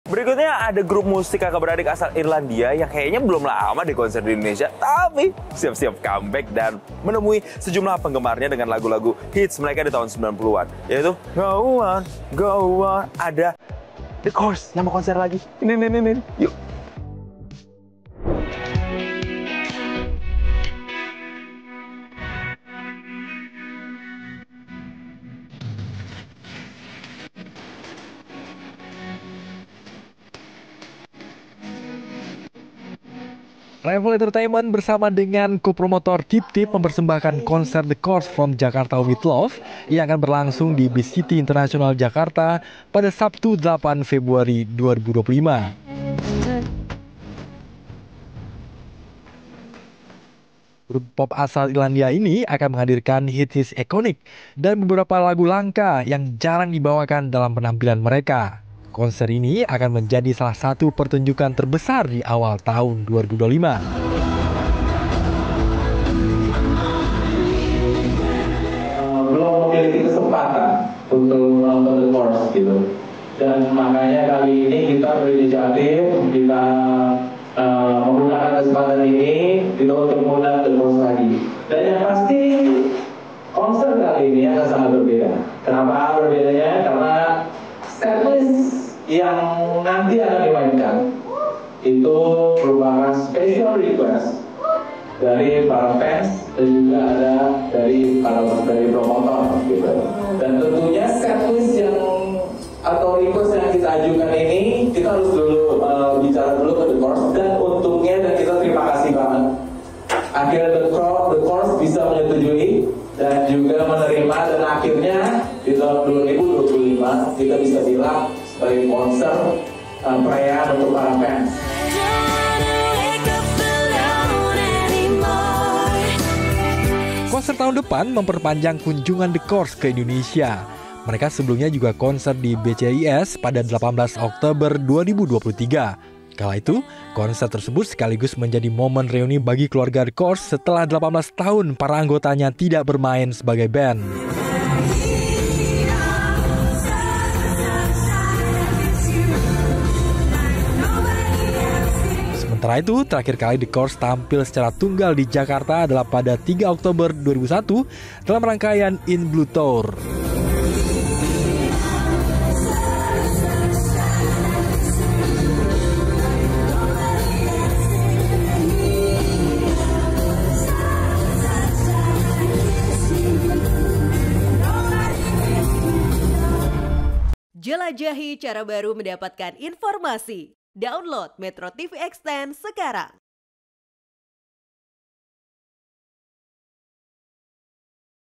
The Corrs Bakal Konser di Jakarta, Bawakan Lagu Lagu Ikonik - [ShowBiz]
MetroTV, The Corrs Bakal Konser di Jakarta, Bawakan Lagu Lagu Ikonik